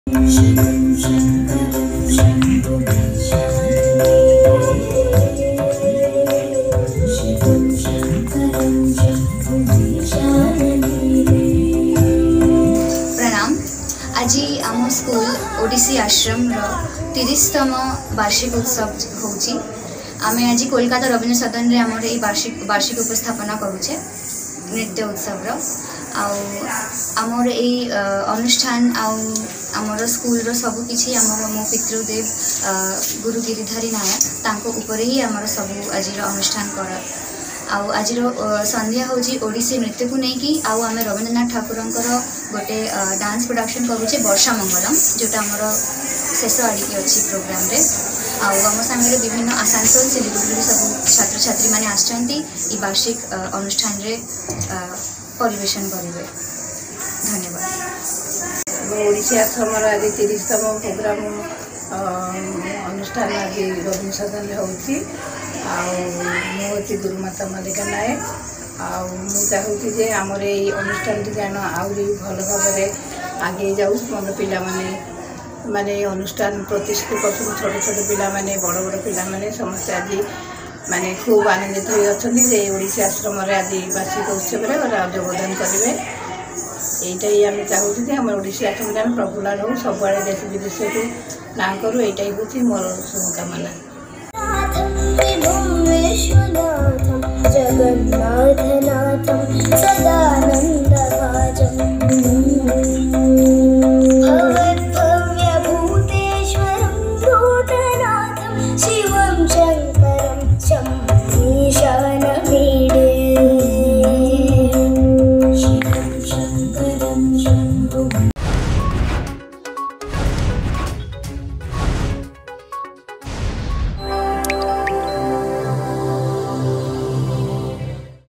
30th Annual Function Organised At Odissi Ashram In Kolkata
30th Annual Function Organised At Odissi Ashram In Kolkata.